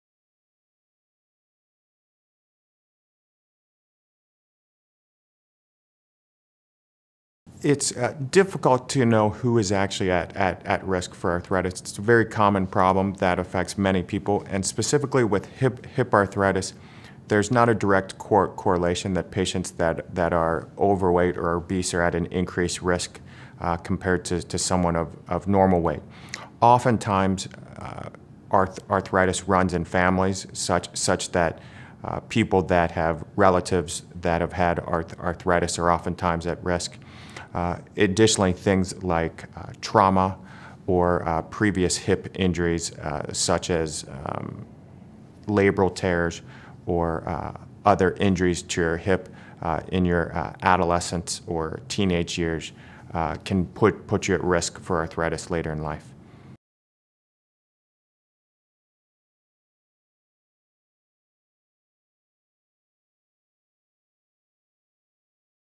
Who is at risk for osteoarthritis?
Dr. Ryan Bauman discusses the risk factors involved in osteoarthritis, the most common type of arthritis.  Find more answers to frequently asked questions about hip arthritis